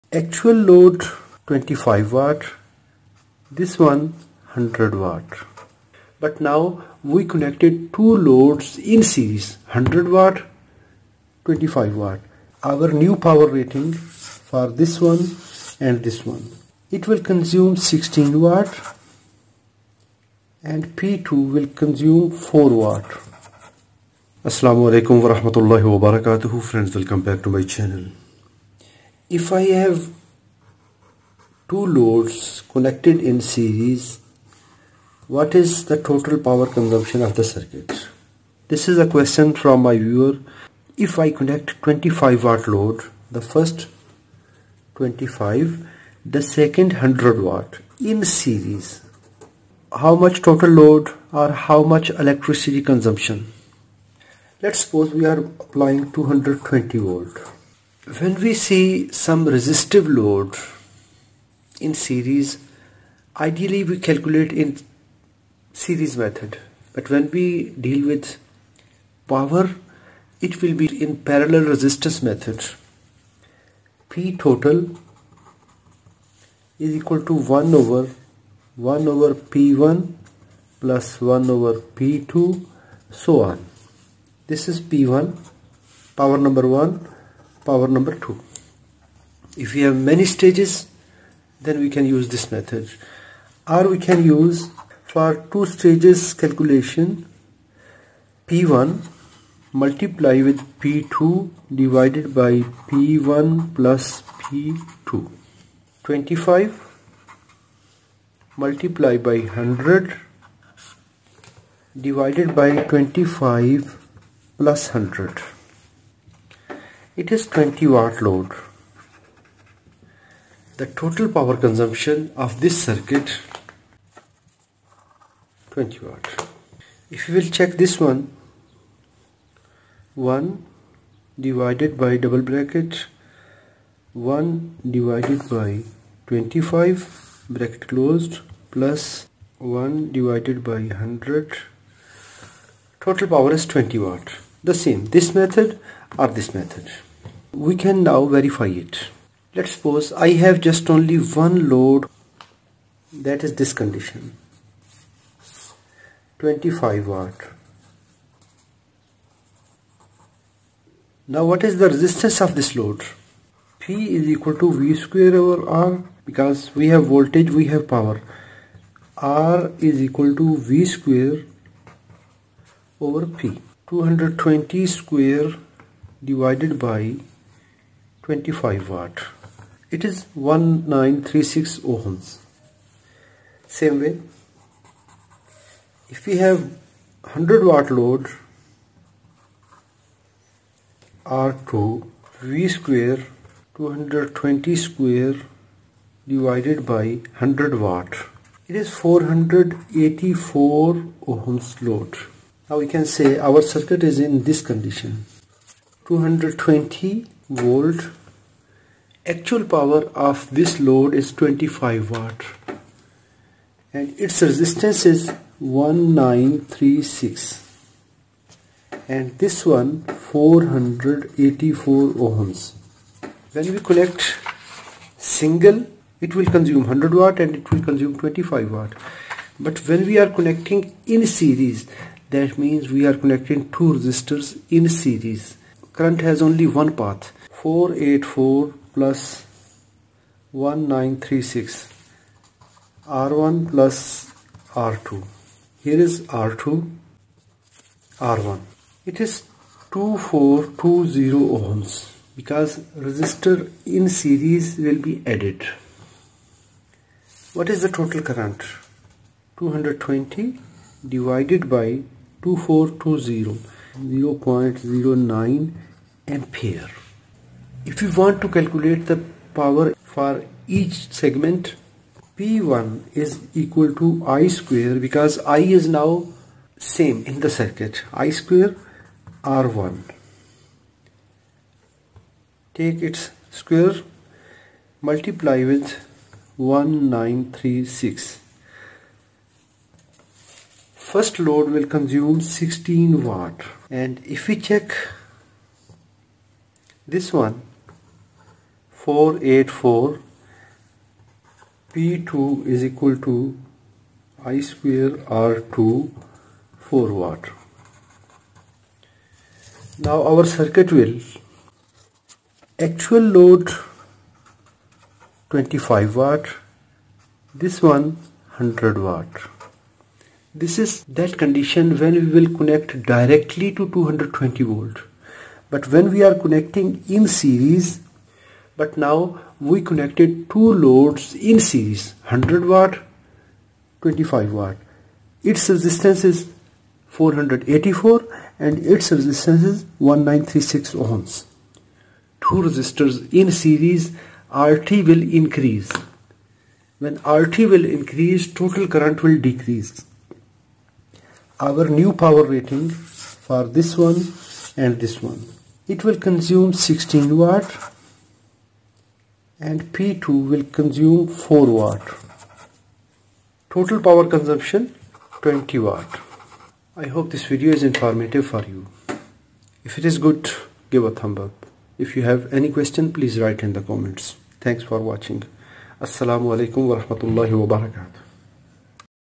{1162} Power calculation in series circuit
In this video number {1162} Power calculation in series circuit. I explained how to calcuate power in series connected loads